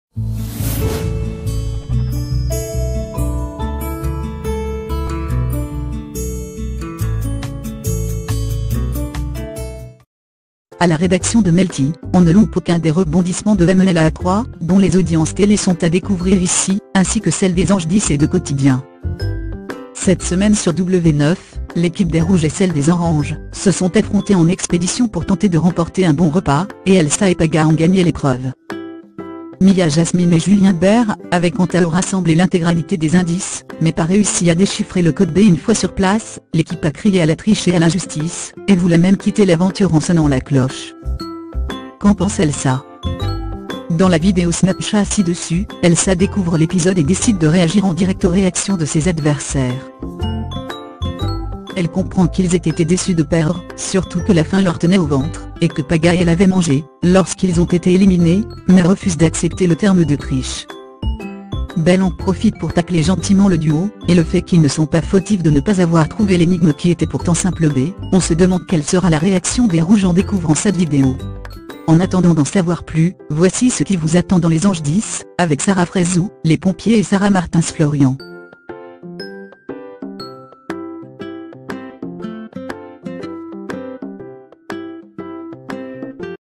MOUNDIR ET LES APPRENTIS AVENTURIERS 3 : ELSA DASC ET PAGA ACCUSÉS DE PAR JULIEN BERT ET MIL
A la rédaction de melty, on ne loupe aucun des rebondissements de MELAA3, dont les audiences télé sont à découvrir ici ainsi que celles des Anges 10 et de Quotidien.
 Cette semaine sur W9, l'équipe des rouges et celle des oranges se sont affrontés en expédition pour tenter de remporter un bon repas, et Elsa et Paga ont gagné l'épreuve.